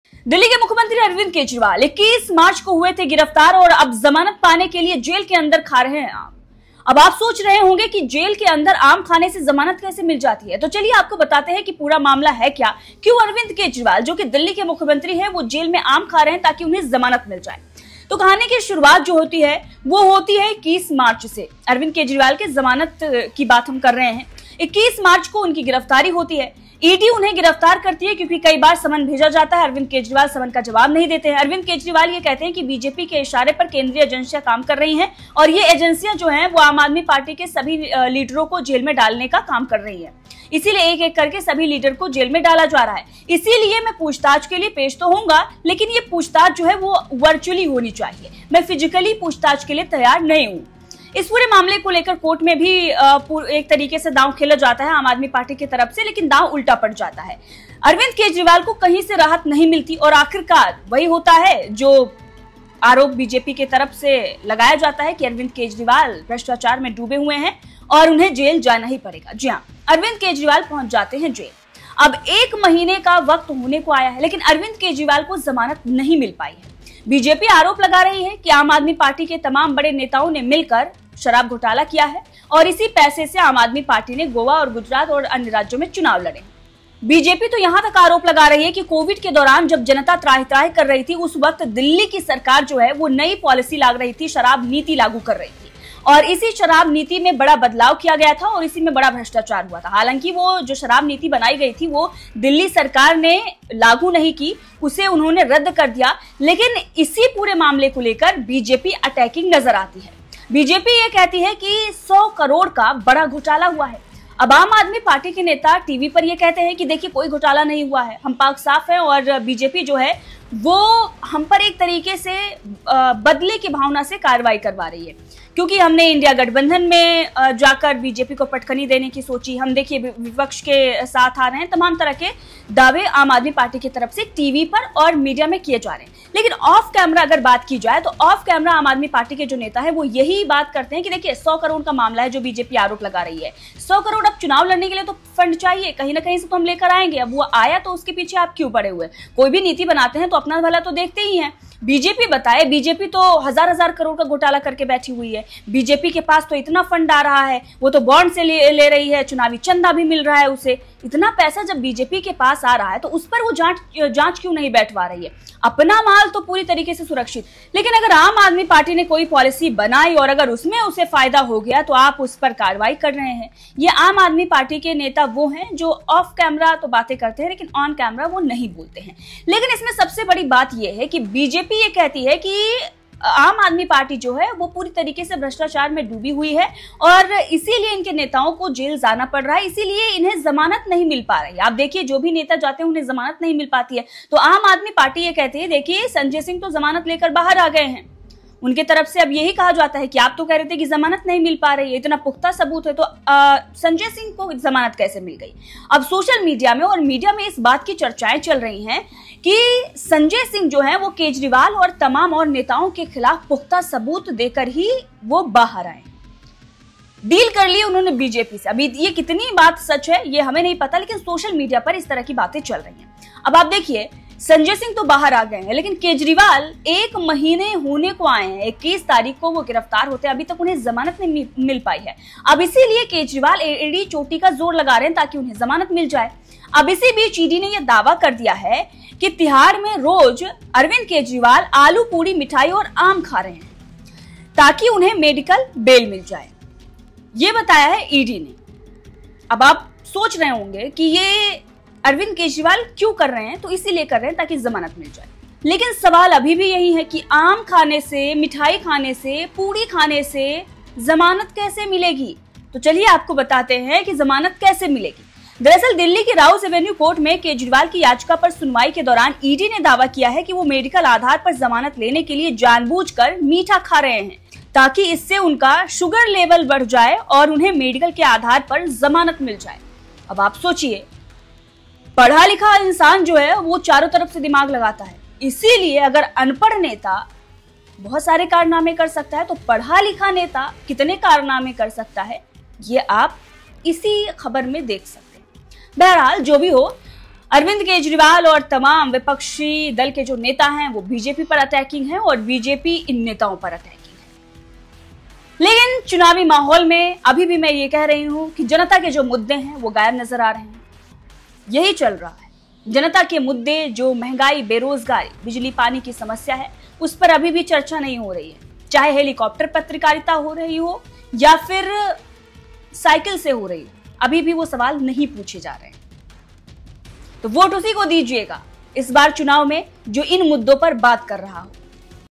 Arvind Kejriwal को Jail में आम खाने से कैसे मिलेगी Bail ? | ED | BJP
Arvind Kejriwal 21 March 2024 को ED के द्वारा Arrest हुए थे तभी से वो Jail में बंद हैं और Bail नहीं मिल पा रही है लेकिन अब Arvind Kejriwal Bail के लिए आम खा रहे हैं। तो चलिए जानते हैं Jail में आम खा कर कैसे मिलेगी Kejriwal को Court से Bail ? .

Anchal Singh